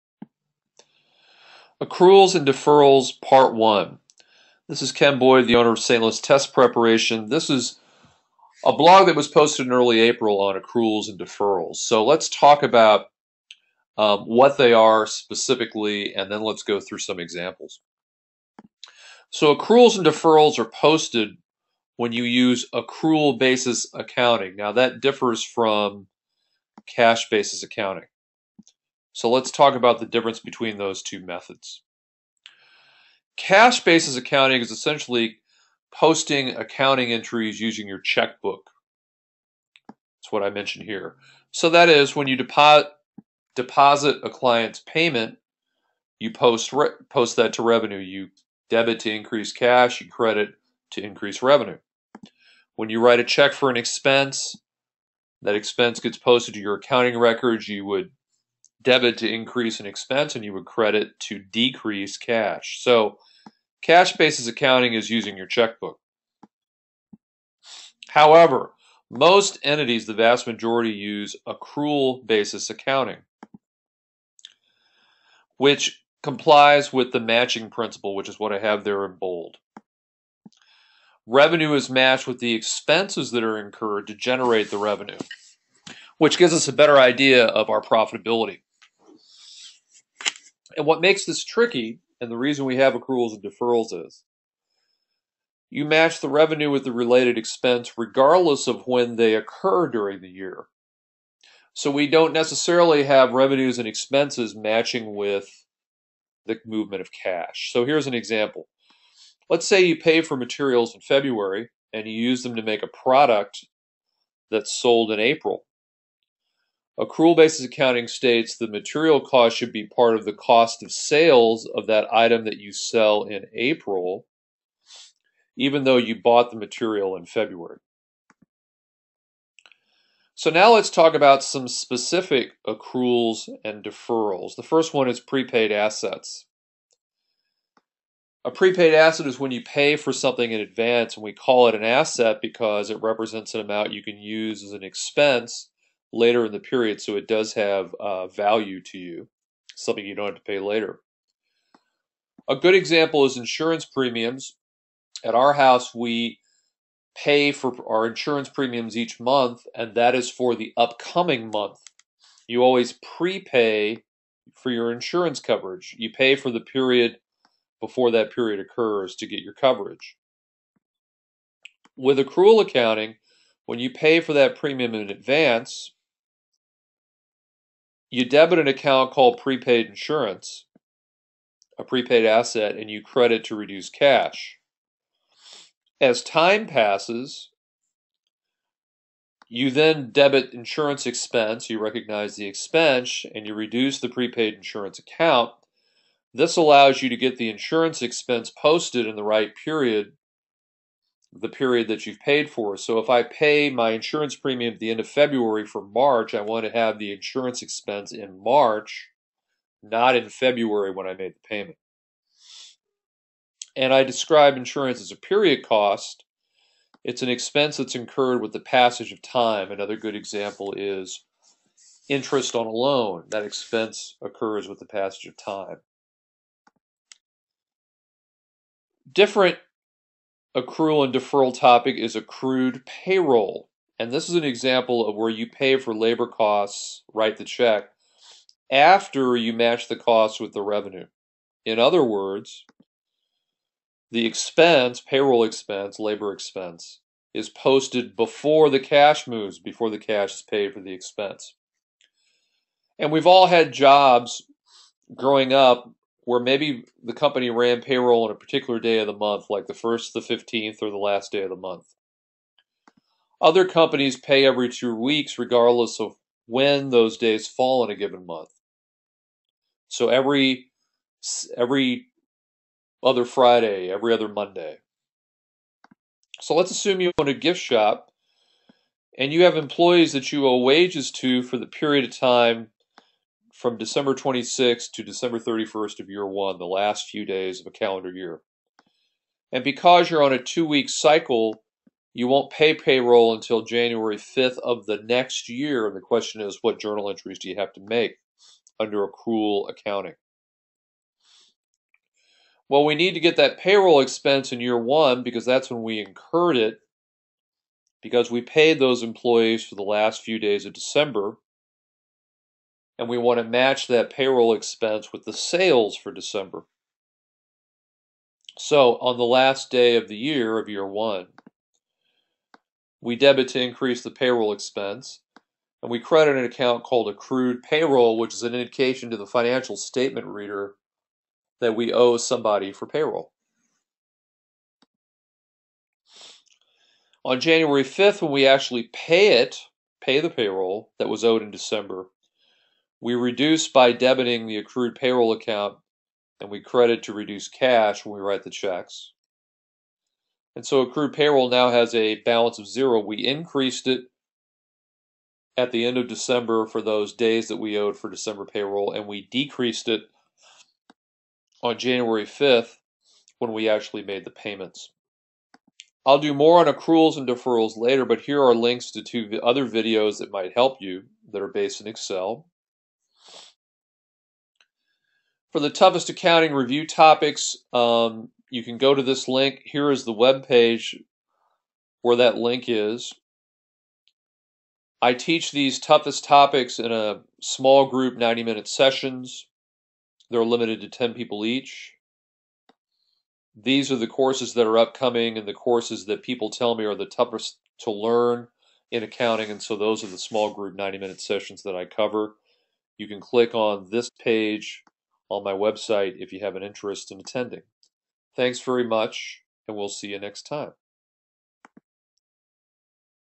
Financial Accounting 5A: Accruals and Deferrals
This Financial Accounting video explains Accruals and Deferrals, accrual basis accounting, prepaid assets, and cash basis accounting.